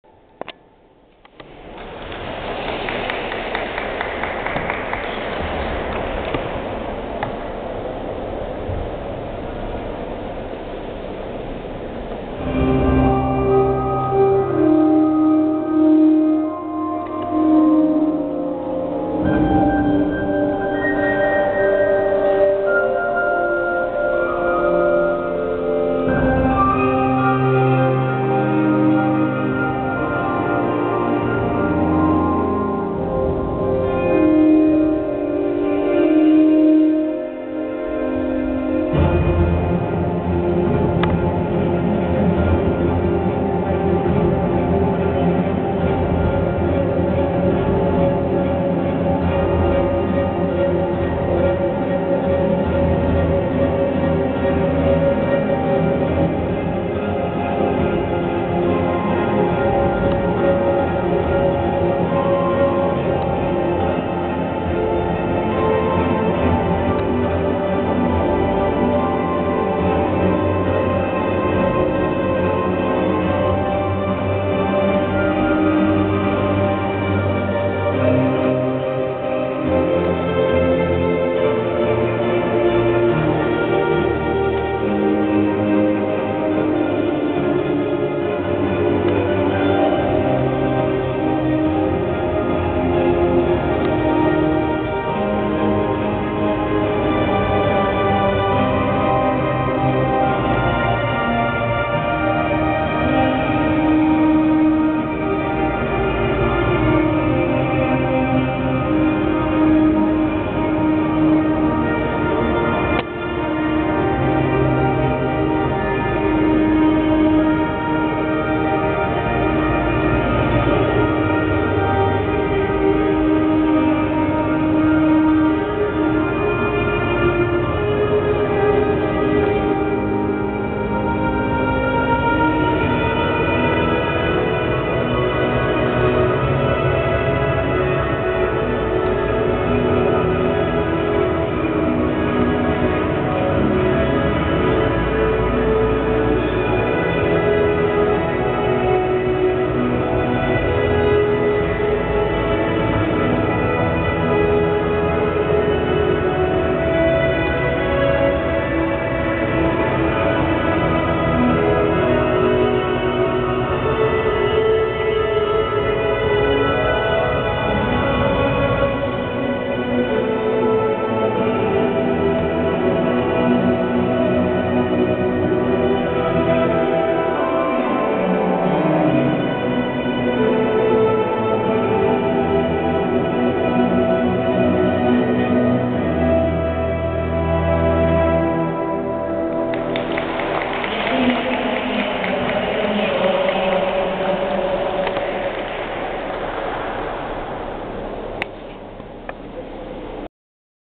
VIII. Veľká cena Ružomberka v krasokorčuľovaní (1)
VIII. Veľka cena Ružomberka v krasokorčuľovaní sa uskutočnila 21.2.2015 na zimnom štadióne v Ružomberku.
VIII. Grand Prix of Figure Skating Ruzomberok took place on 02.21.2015 ice rink in the castle.